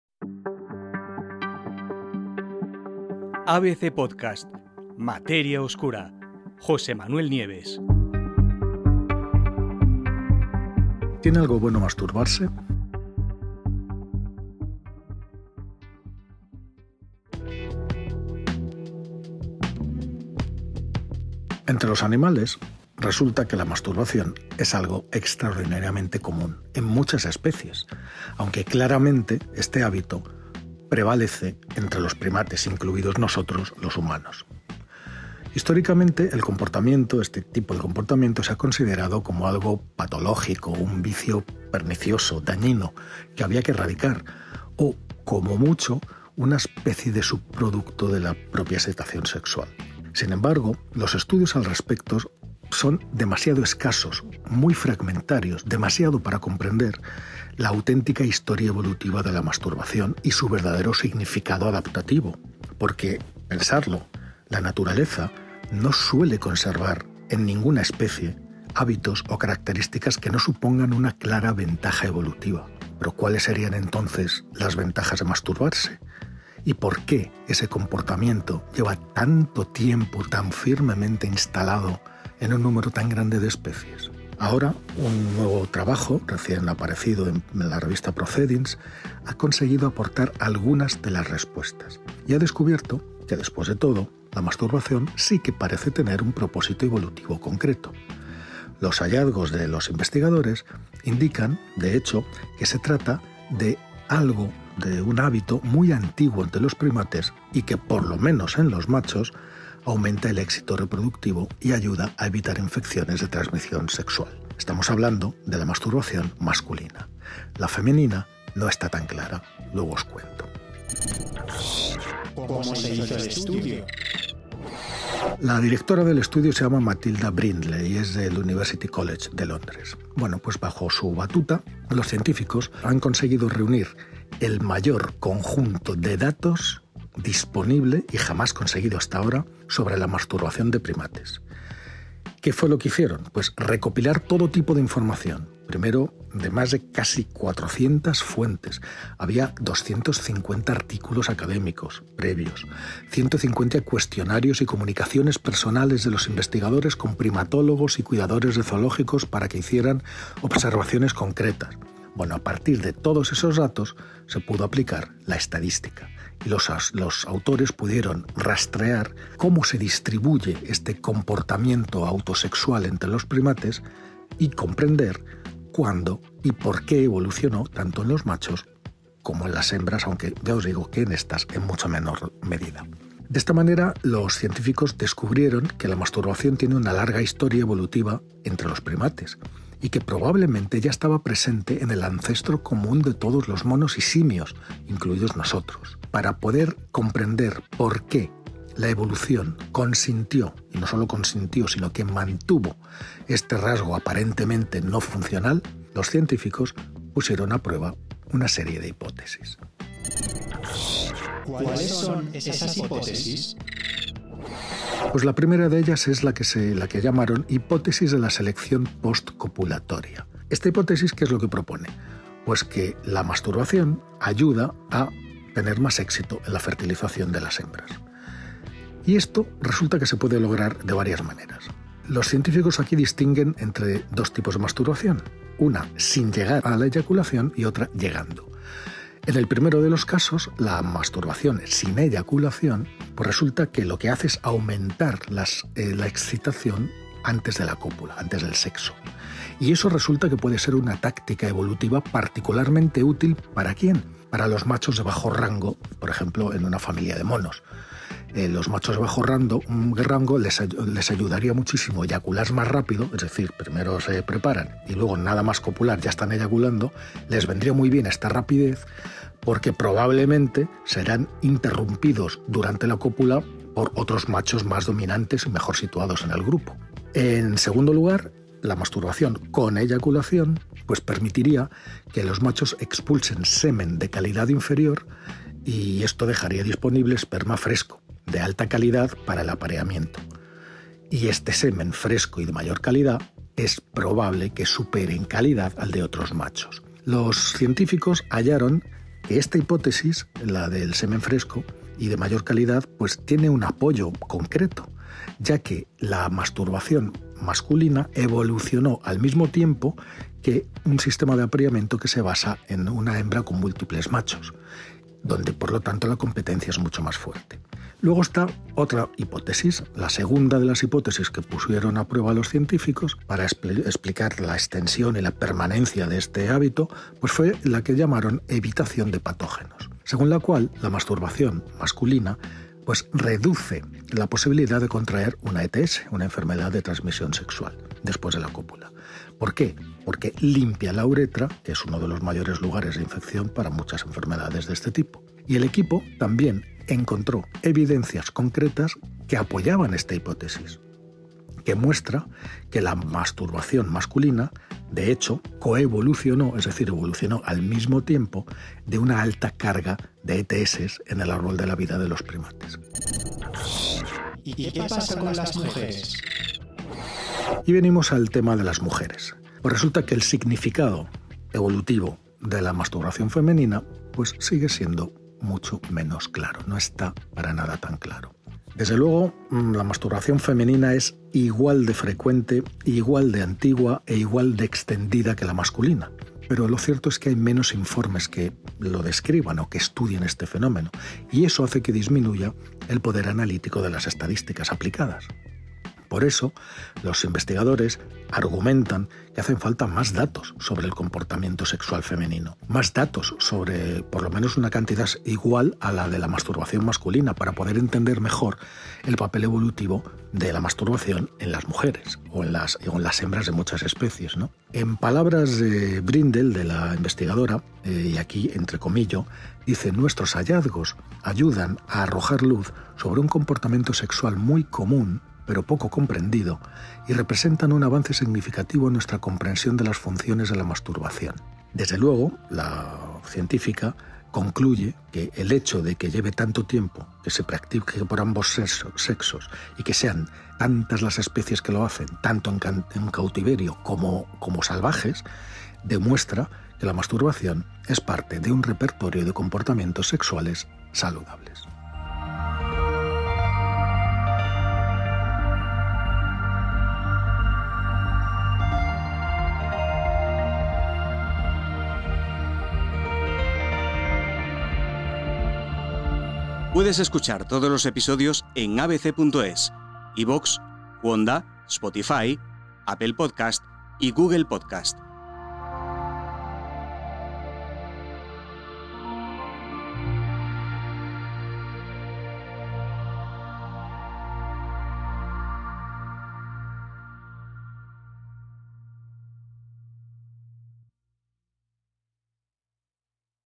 Podcast | ¿Tiene algo bueno masturbarse?
En el reino animal, la masturbación es algo muy común, aunque prevalece claramente entre los primates, incluidos los humanos. Históricamente, este comportamiento se consideraba como algo patológico, un vicio pernicioso que había que erradicar o, como mucho, un subproducto de la excitación sexual.

Sin embargo, los estudios al respecto son demasiado escasos y fragmentados para comprender la historia evolutiva de la masturbación y su verdadero significado adaptativo. La naturaleza no suele conservar en las especies hábitos o características que no supongan una clara ventaja evolutiva. ¿Pero cuáles serían entonces las ventajas de masturbarse? ¿Y por qué ese comportamiento lleva tanto tiempo firmemente instalado en un número tan grande de especies?

Ahora, una nueva investigación recién publicada en 'Proceedings of The Royal Society B' ha conseguido aportar algunas respuestas y ha descubierto que, después de todo, la masturbación parece tener un propósito evolutivo muy concreto. Los hallazgos, de hecho, indican que se trata de un rasgo muy antiguo en los primates y que, por lo menos en los machos, aumenta el éxito reproductivo y ayuda a evitar infecciones de transmisión sexual.